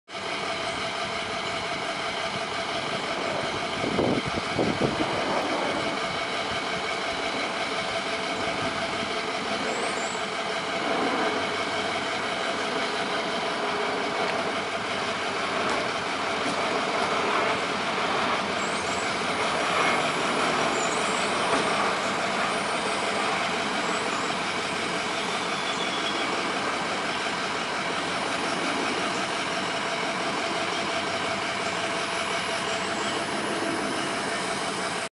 BGE changes street light bulb #4
BGE changes street light fixture and bulb